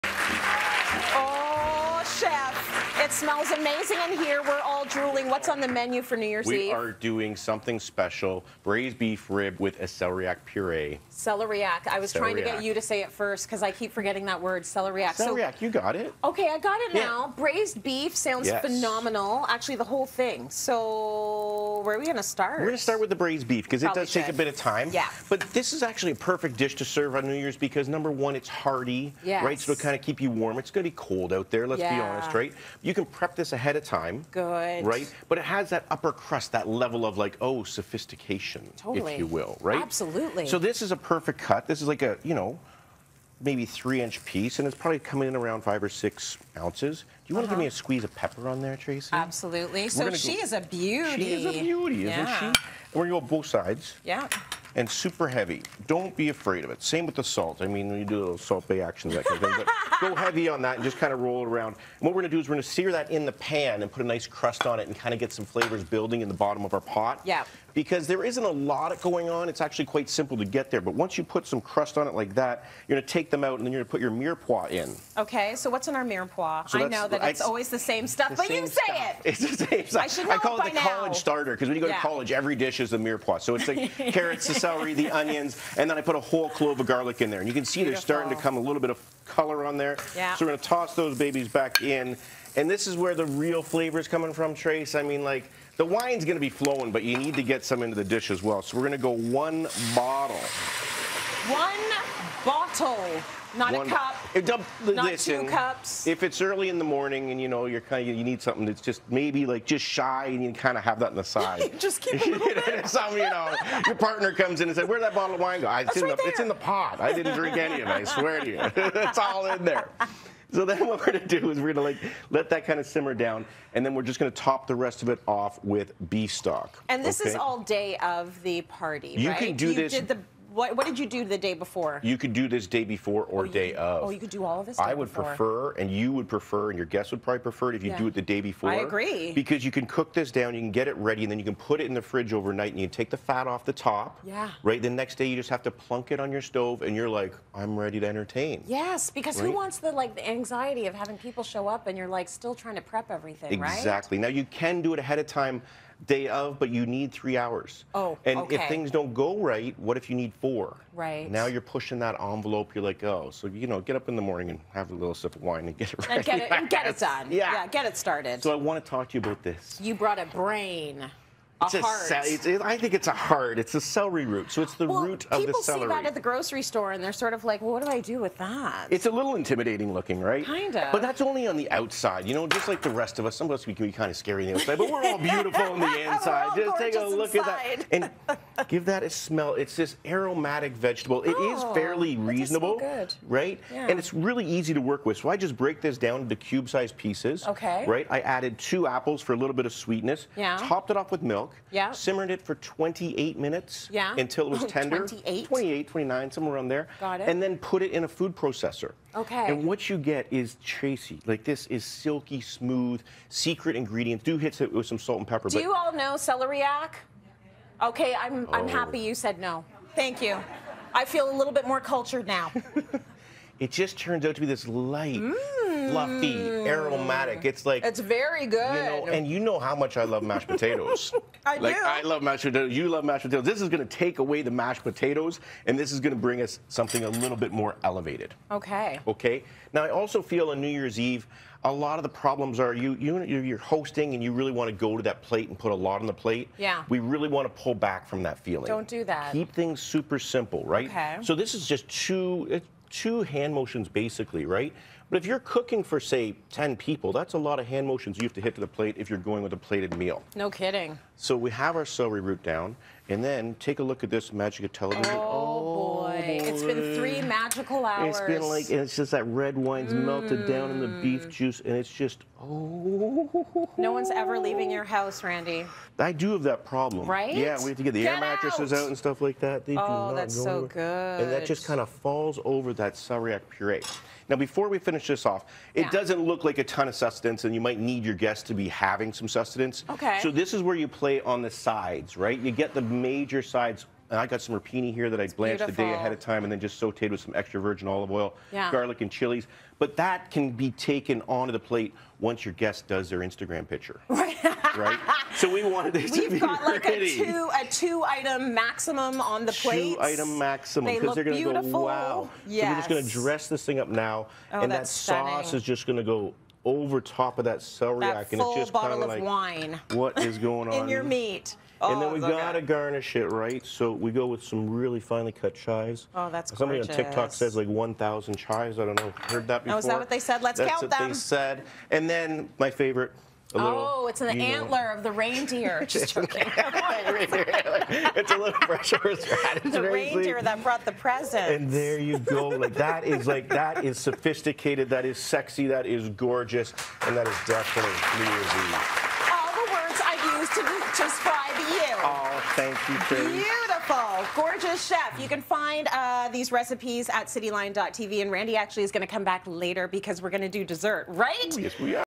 Braised short ribs with celeriac puree
Chef Randy Feltis shows how to make a drool-worthy braised short rib dinner to surprise your guests with at your next gathering. "No one's ever leaving your house."